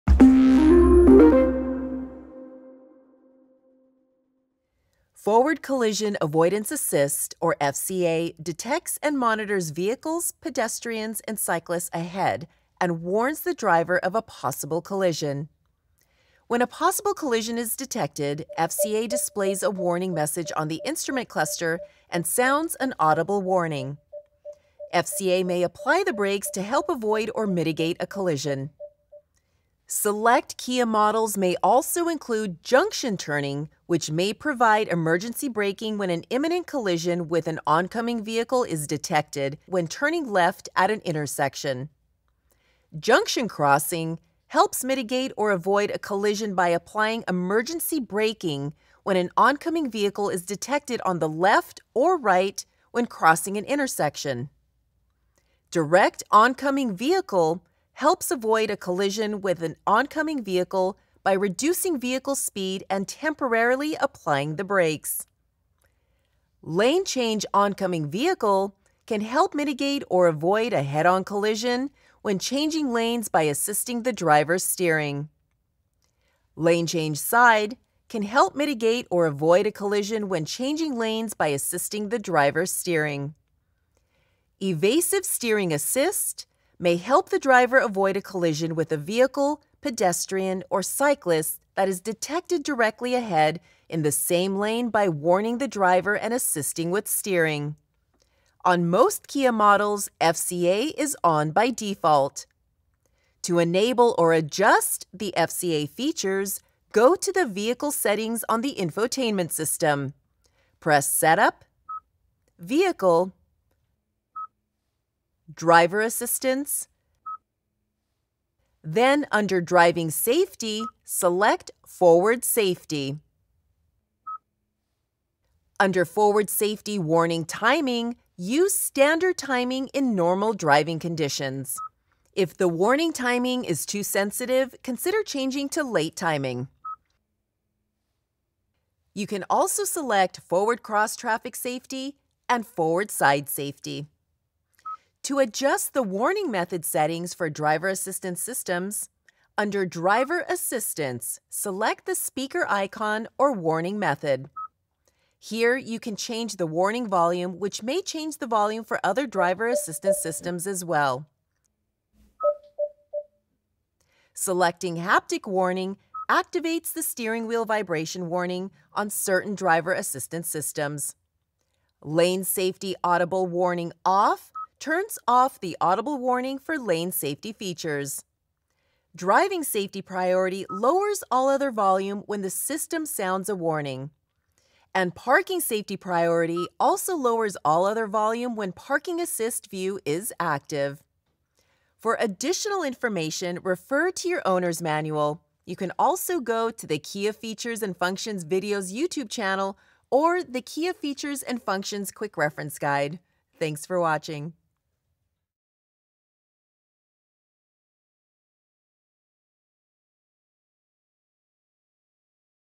Forward Collision Avoidance (FCA) - Advanced Features
FORWARD COLLISION-AVOIDANCE ASSIST, or FCA, detects and monitors vehicles, pedestrians, and cyclists ahead and warns the driver of a possible collision. When a possible collision is detected, FCA displays a warning message on the Instrument Cluster and sounds an audible warning.
FCA may apply the brakes to help avoid or mitigate a collision.
Select Kia models may also include JUNCTION TURNING, which may provide emergency braking when an imminent collision with an oncoming vehicle is detected in an adjacent lane, when turning left at an intersection.
JUNCTION CROSSING helps mitigate or avoid a collision by applying emergency braking when an oncoming vehicle is detected on the left or right, when crossing an intersection.
DIRECT ONCOMING VEHICLE helps avoid a collision with an oncoming vehicle by reducing vehicle speed and temporarily applying the brakes.
LANE-CHANGE ONCOMING VEHICLE can help mitigate or avoid a head-on collision when changing lanes by assisting the driver’s steering.
LANE-CHANGE SIDE can help mitigate or avoid a collision when changing lanes by assisting the driver’s steering.
EVASIVE STEERING ASSIST may help the driver avoid a collision with a vehicle, pedestrian or cyclist that is detected directly ahead in the same lane by warning the driver and assisting with steering.

On most Kia models, FCA is on by default. To ENABLE OR ADJUST the FCA features, go to the Vehicle Settings on the Infotainment System: Press SETUP, VEHICLE, DRIVER ASSISTANCE then under DRIVING SAFETY, select FORWARD SAFETY.

Under FORWARD SAFETY WARNING TIMING, use Standard Timing in normal driving conditions. If the Warning Timing is too sensitive, consider changing to Late timing. You can also select FORWARD CROSS-TRAFFIC SAFETY and FORWARD/SIDE SAFETY.

To adjust the WARNING METHOD settings for driver assistance systems: Under DRIVER ASSISTANCE, select the speaker icon or WARNING METHOD.
Here you can change the WARNING VOLUME, which will change the warning volume for other driver assistance systems as well. Selecting “Haptic Warning” activates the steering wheel vibration warning on certain driver assistance systems. “Lane Safety Audible Warning Off” turns off the audible warning for Lane Safety features “Driving Safety Priority” lowers all other audio volume when the Driving Safety system sounds a warning.

For additional information, refer to your Owner’s Manual. You can also go to the Kia Features and Functions Videos YouTube channel or the Kia Features and Functions Quick Reference Guide.